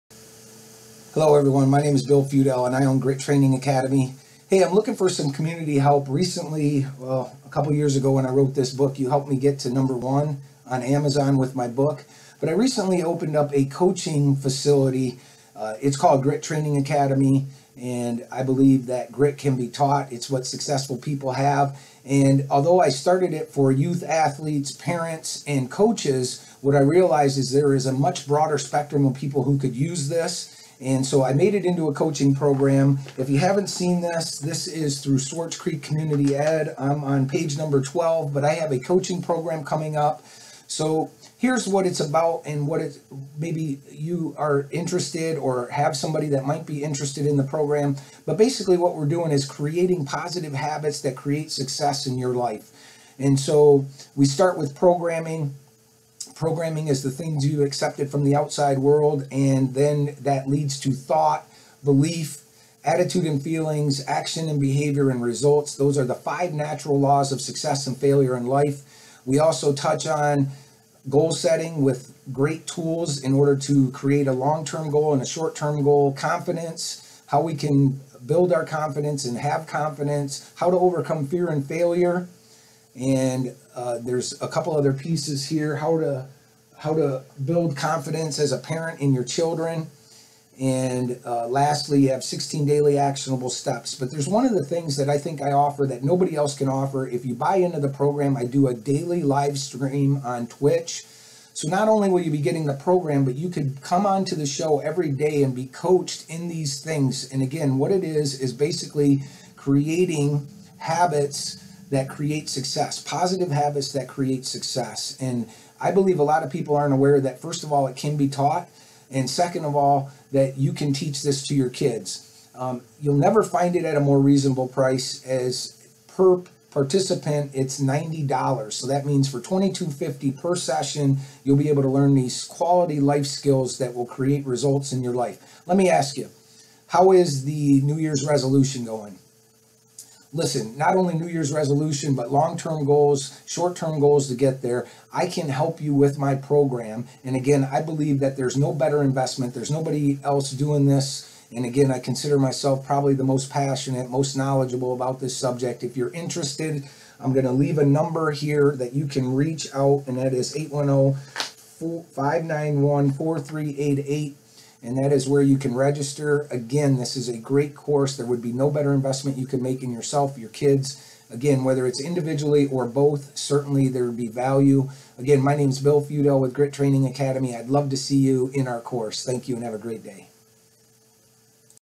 Success Coaching With Grit Training Academy
Grit Training Academy is presenting a 4 week coaching program that creates succes in Sports, Business and Life.

Do you have a process that creates succes in your life or are you leaving that to chance?

 The most succesful people on the planet have this thing called GRIT, cool thing is it is coachable.

Are you aware of your programming? How did it happen and how you can change it.

The five natural laws of success or failure/

Goal setting: long and short term and the rules for setting goals and the tools to get there.

Would you like more confidence?

 Learn how to overcome fear and failure to create the success you deserve?

What is massive Action and what does it take?

Do you have a process that creates results in your life?

This course covers all that and more although created for youth athletes, parents and coaches it has a much larger audience.

People need this information it is everything hoding you back from your Success.

The single most important thing holding you back from everything you ever wanted in life is YOU.